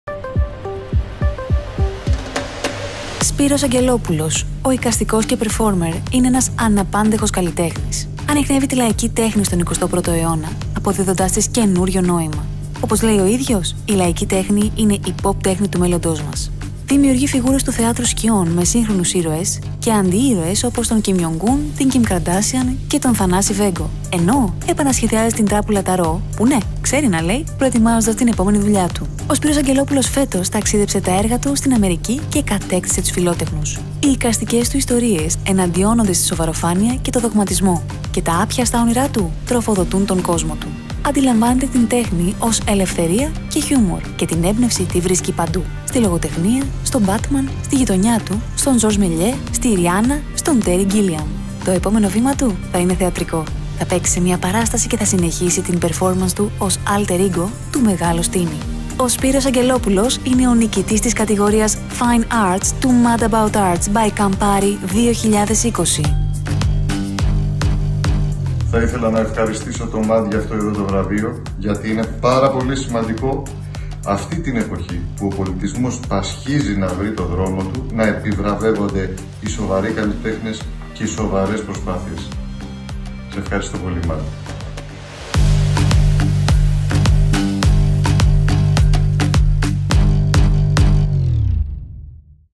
MAD ABOUT ARTS by CAMPARI - Fine Arts Spiros Aggelopoulos
Κάνει τη λαϊκή τέχνη POP με ήρωες όπως η Κιμ Καρντάσιαν και ο Θανάσης Βέγγος, ενώ οι δημιουργίες του εναντιώνονται στη σοβαροφάνεια και τον δογματισμό.
Αυτός είναι και ο λόγος που ο Σπύρος Αγγελόπουλος κατέκτησε το βραβείο Fine Arts των Mad About Arts by Campari!